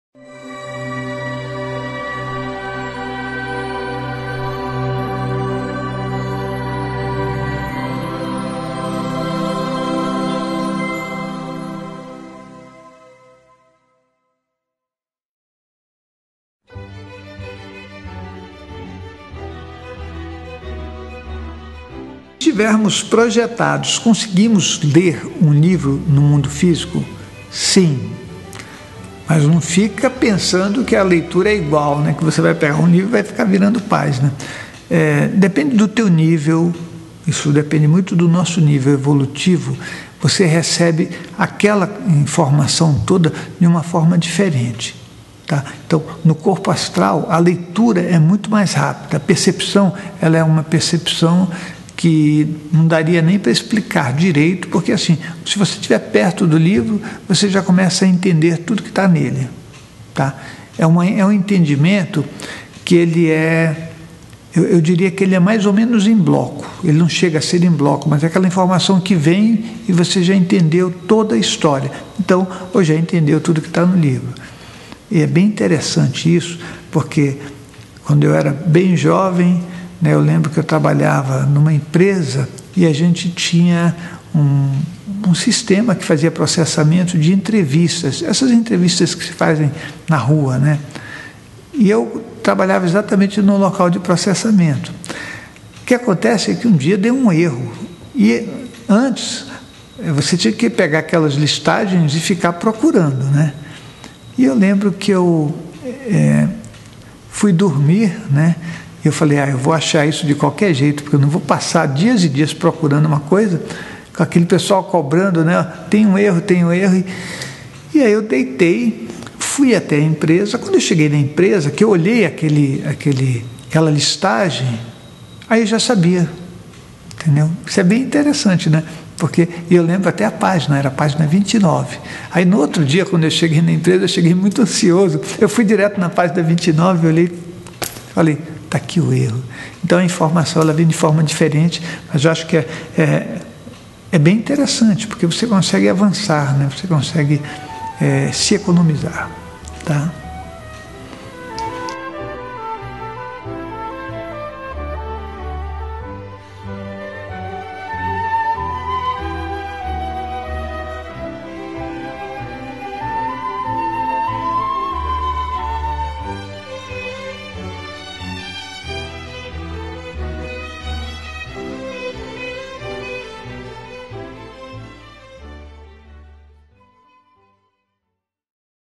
É possivel ler um livro em projeção astral?
É possível acessar o conteúdo de um livro através da projeção astral? Essa é a pergunta respondida por Moisés Esagui nesse vídeo-resposta.